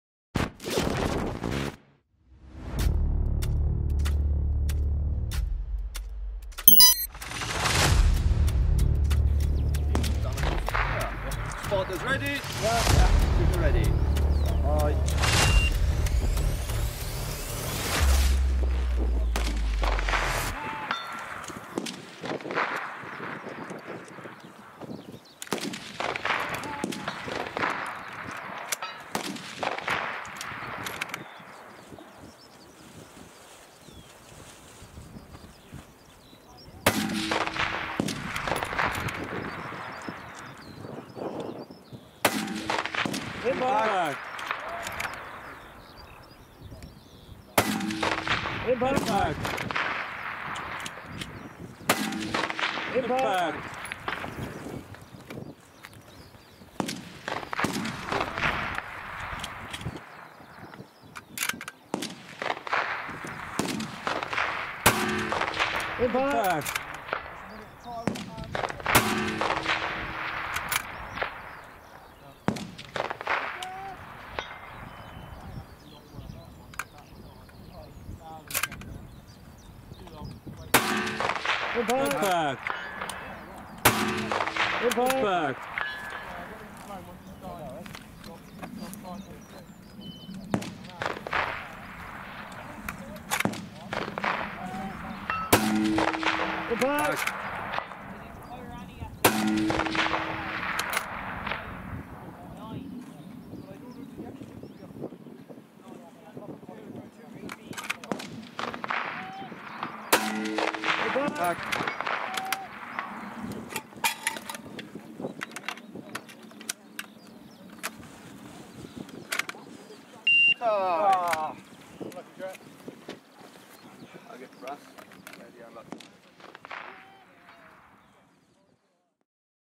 Tyre Podium, stage 4 PRS UK match
Stage 4 from last weekend.
Lots of the stages had some really confusing target orders, I can only conclude that Precision Rifle Series UK just hates us.
Annoyed I had a couple of fumbles with a stoppage and my magazine, I could have had a much higher score. That's one to dry fire.

Using the new Zerotech trace ADV